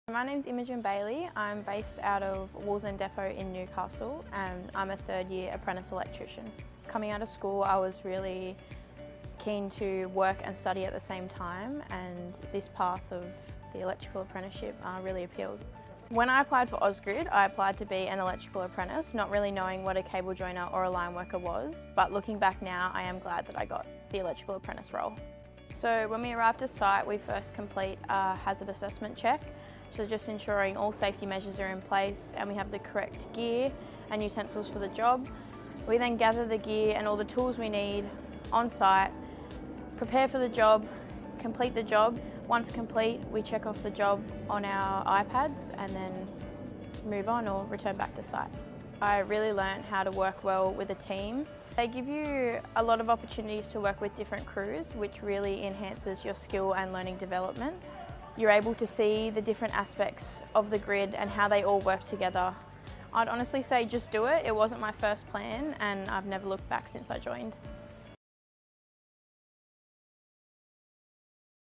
Imogen's Story
Imogen gives our fast fire Q&A her best shot as she tells us about her experiences as an Electrician in the Bright Spark Apprentice Program.